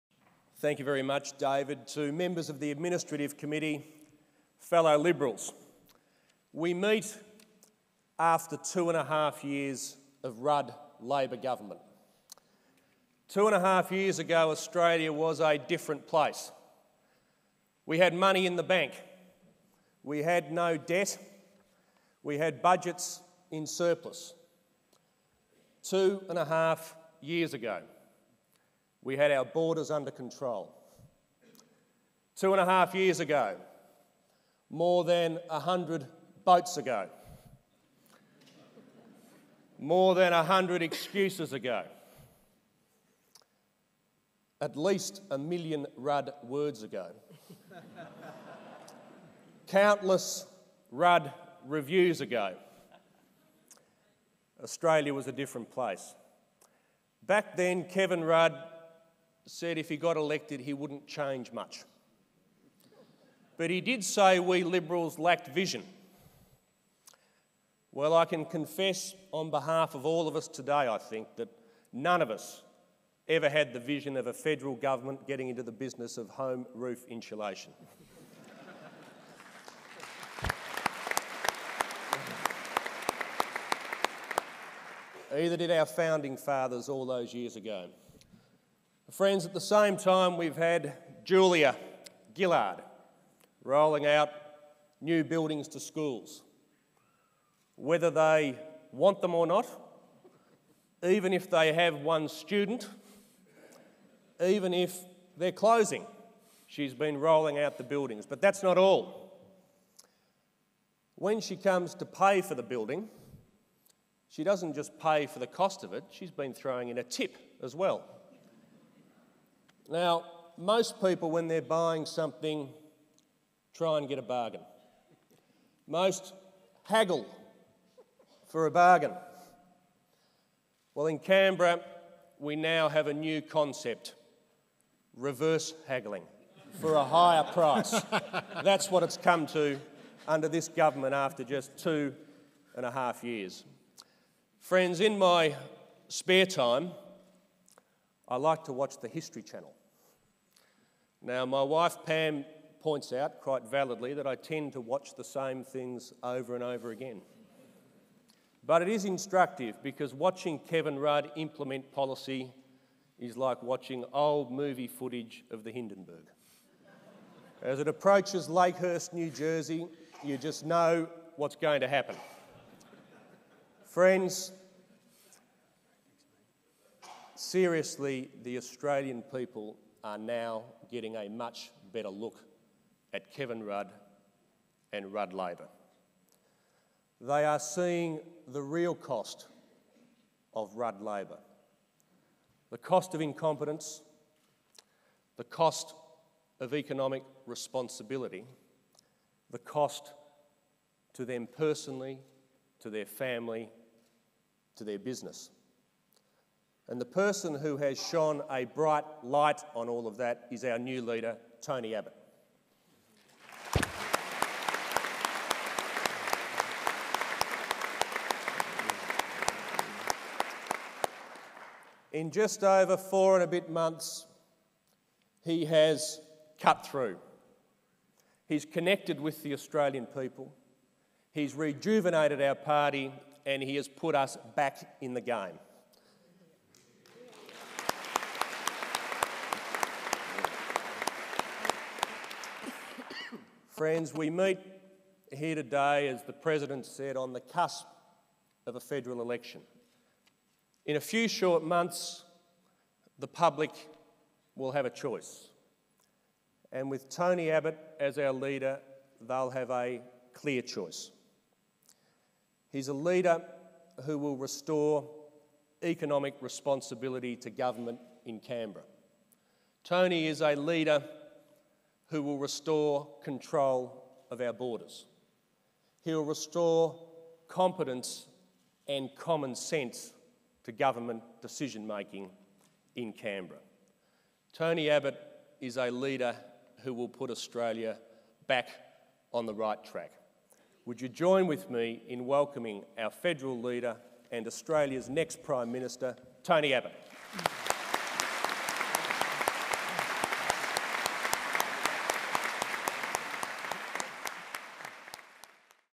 Tony Smith introducing Tony Abbott - Liberal Party State Council, April 2010
Tony Smith introduced Tony Abbott at the 151st State Council of the Victorian Division of the Liberal Party.

Australians face a clear choice at the next election - Kevin Rudd, who is all talk and no action - or the Liberal Party and Tony Abbott - who stand for real, direction action on getting our borders under control, better standards in education and managing our economy.